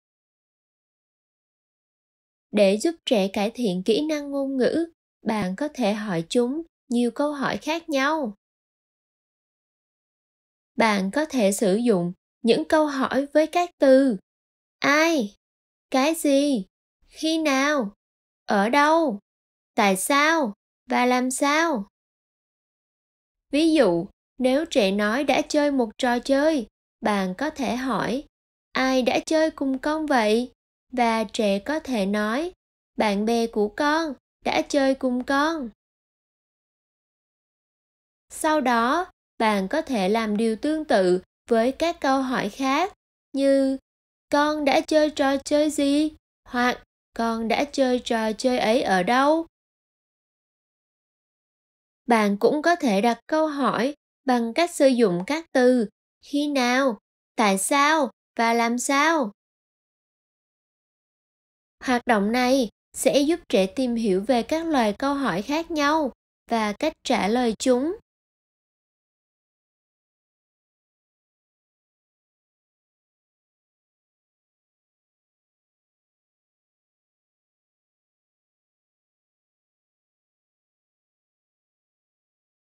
Các từ dùng để hỏi - Question Words (Vietnamese)
Bạn có thể giúp trẻ cải thiện kỹ năng ngôn ngữ bằng cách đặt những câu hỏi với các từ ai, cái gì, khi nào, ở đâu, tại sao và làm sao.

Vietnamese translation by Huyen Huyn and Anh Le. Narration by Anh Le.

You can help a child improve language skills by asking questions with the words who, what, when, where, why, and how.